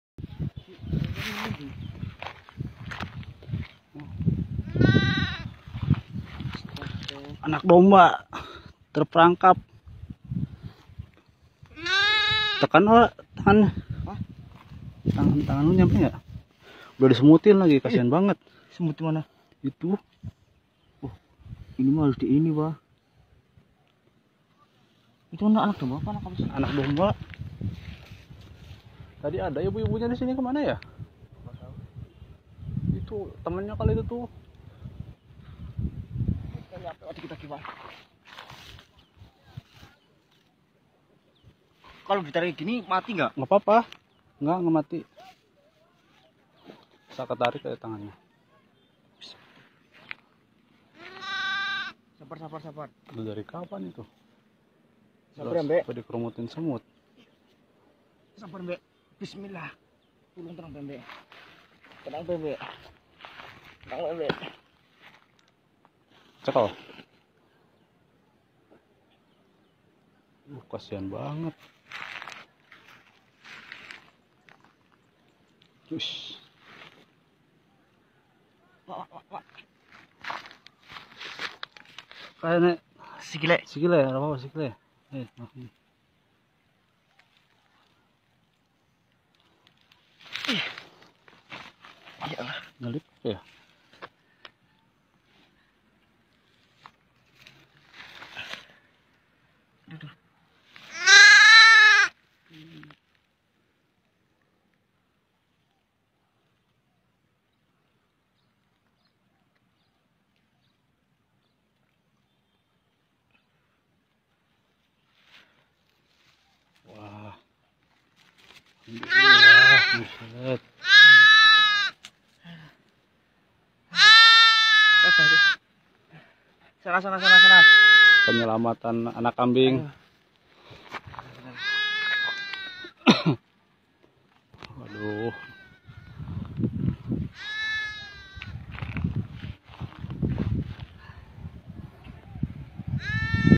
Saat berburu nemu Anak Kambing di Tanah Belah//kita selanatkan
gasengaja saat berburu nemu anak kambing ditanah belah gatau punya siapa kita selamatkan saja.